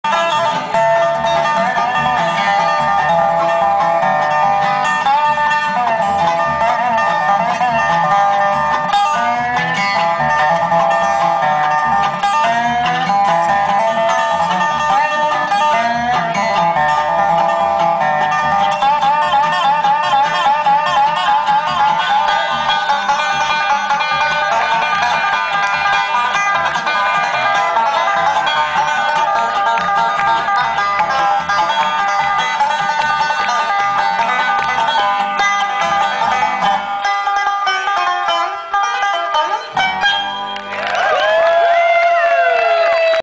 Earls of Leicester(by Angie Doyle) 9/26/15
Stacy and I went to see them near E town..  at Vine Grove Ky. Bluegrass Festival.  It was a great show...Love these guys!!